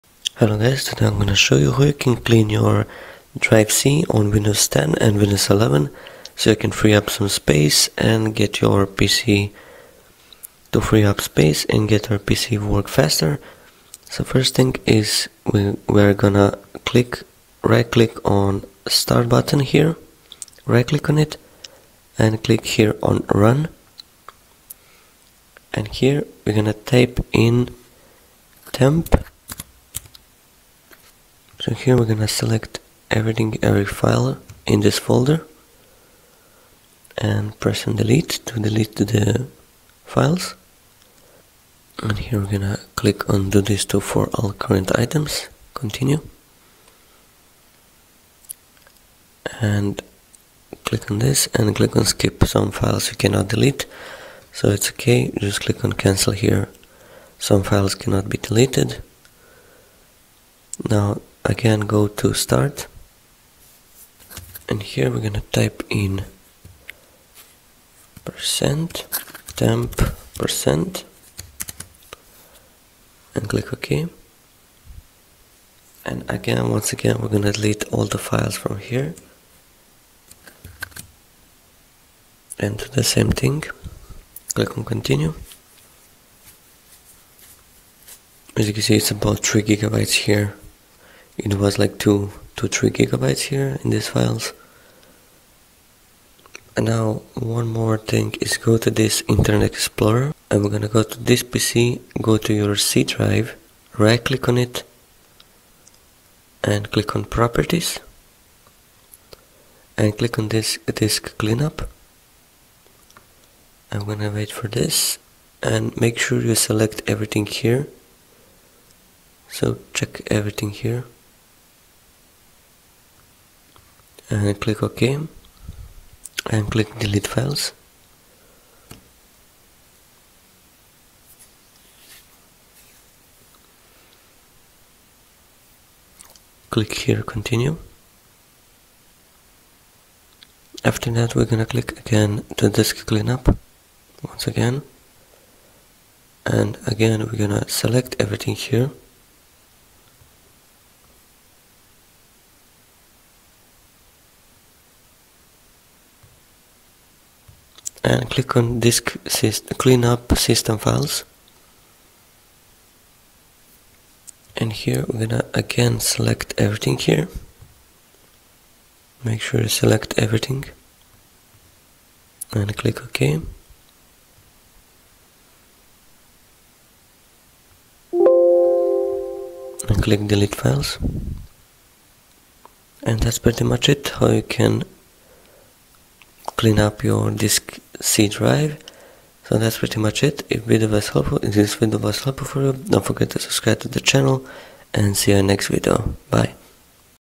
How to Clean C Drive In Windows 10 and 11 (Make Your PC Faster and Smoother)
You can turn automatic subtitles ON on youtube player.